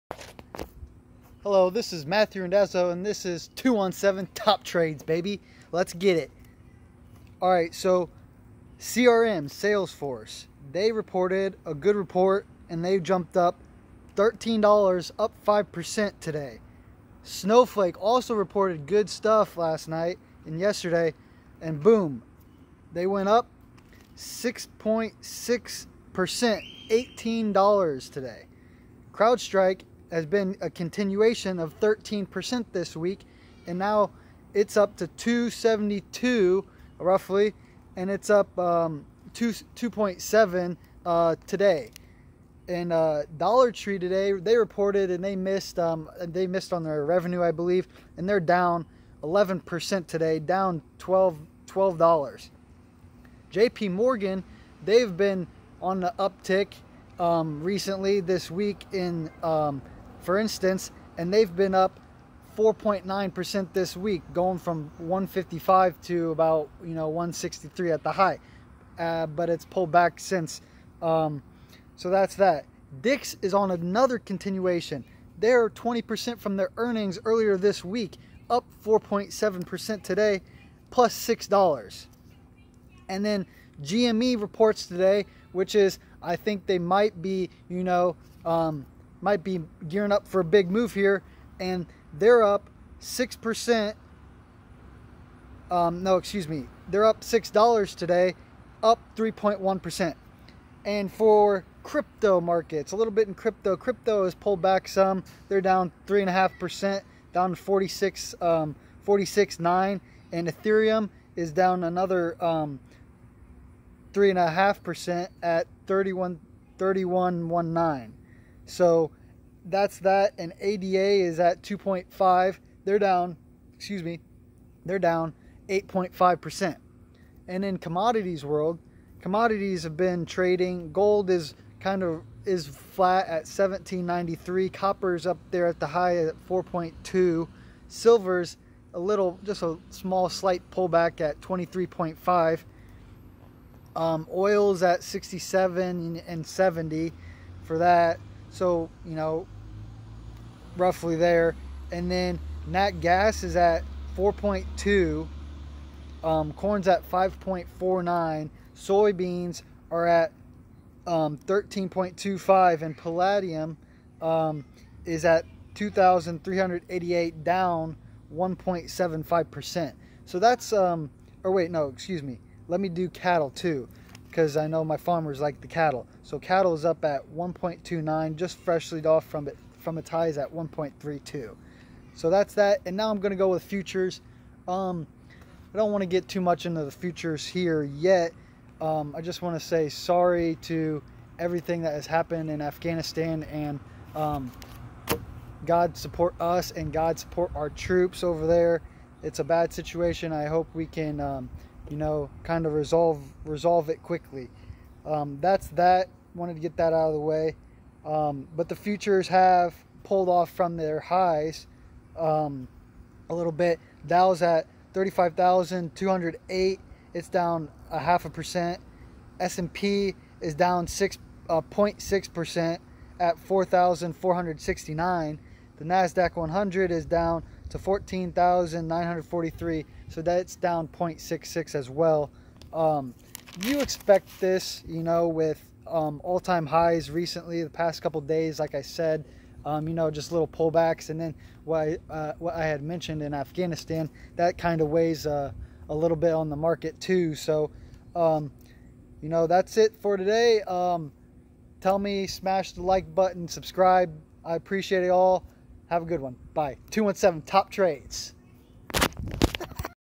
Market Midday 8-26 📈📉 easing off ATH's for S&P500 Nasdaq100 On Afghanistan debacle weighs
stocks commodities crypto futures